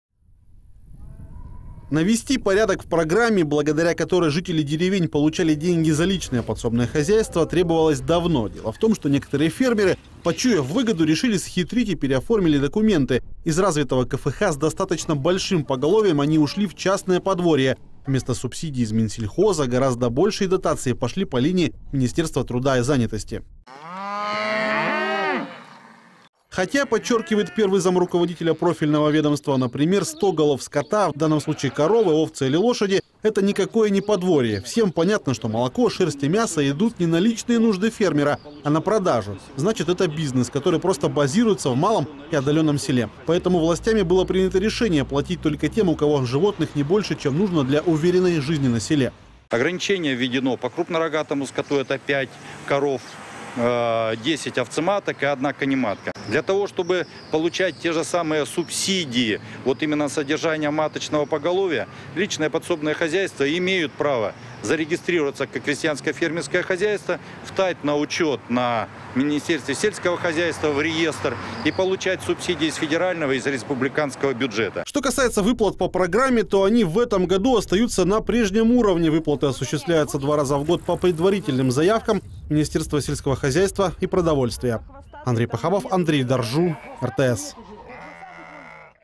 Выплаты за домашний скот: Минсельхоз Хакасии опроверг слухи
Выплаты за маточное поголовье лошадей, овец и коров - будут. Сегодня в министерстве сельского хозяйства прокомментировали сообщение о том, что власти республики прекратили финансирование программы сохранения и развития малых и отдаленных сел. В ведомстве отмечают: эта информация в корне неверная - люди так же могут получить через центр занятости деньги за свой скот. Но с одном условием.